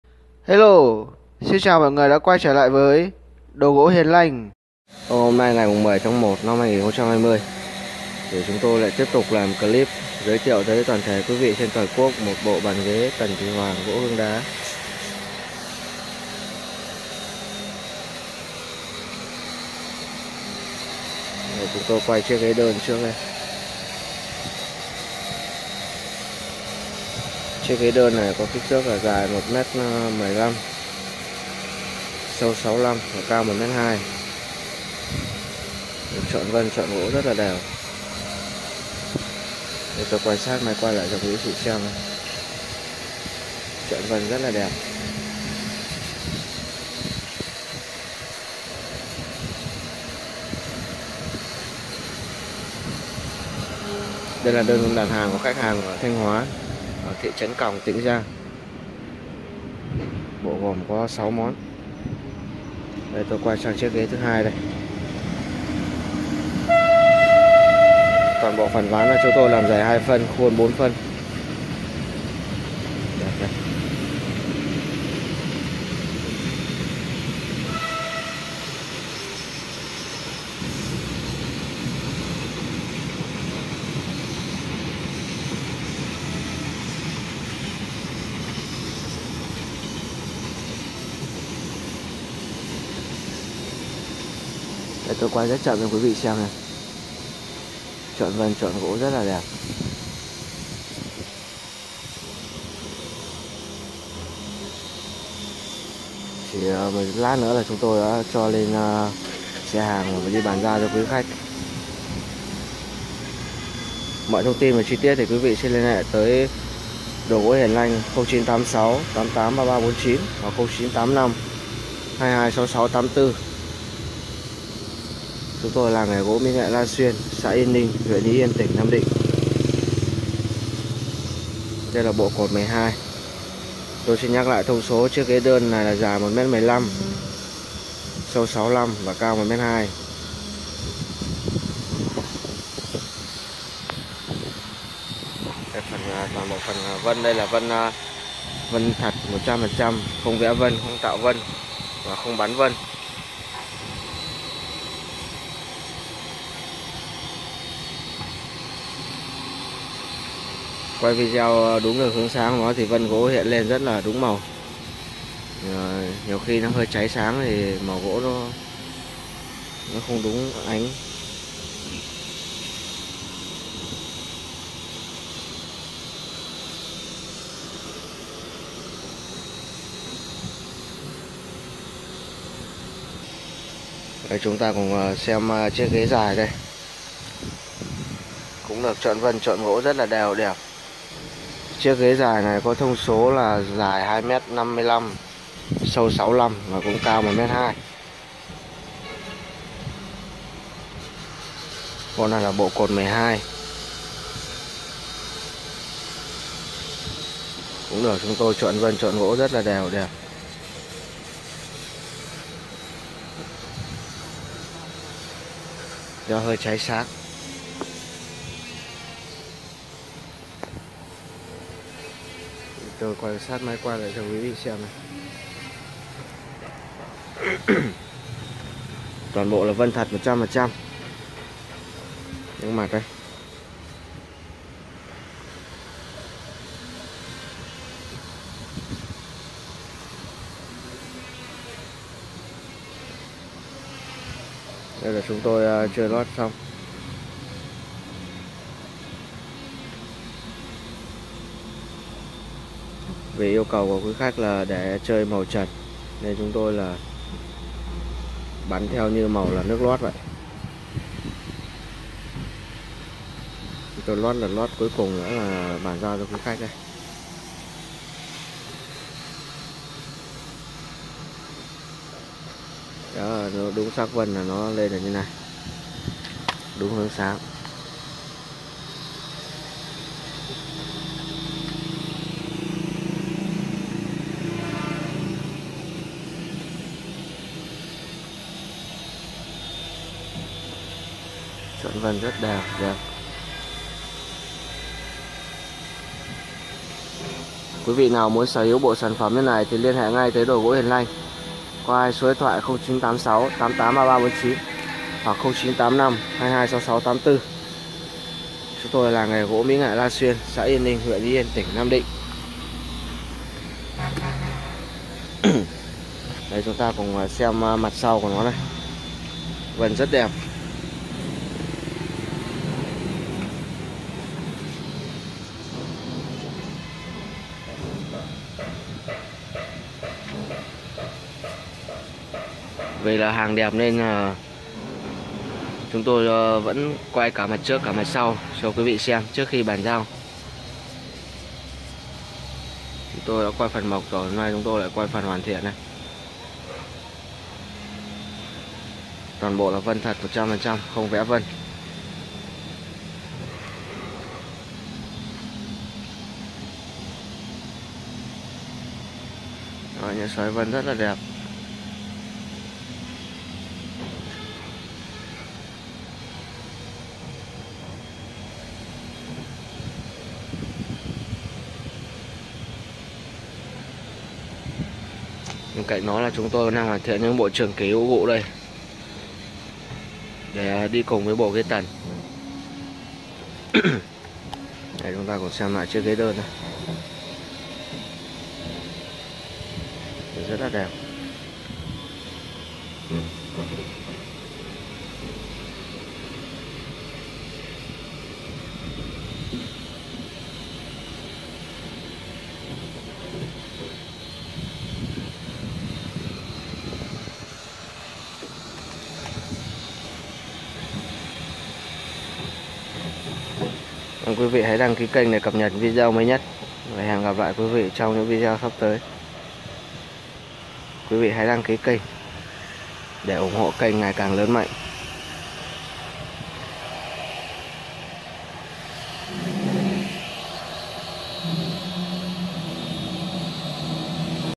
Bộ bàn ghế phòng khách gỗ hương đá đẹp chọn Vân làm Mưa Gió MXH. đồ gỗ hiền lanh.0986883349
Bộ bàn ghế phòng khách gỗ hương đá chọn Vân làm Mưa Gió MXH. đồ gỗ hiền lanh.0986883349
- Bộ bàn ghế phòng khách gỗ hương đá, tần thủy hoàng, cột 12 x 6 món( 1 đoản dài , 2 ghế đơn, 1 bàn to, 1 đôn, 1 bàn trà) - hàng đặt khách Thanh Hóa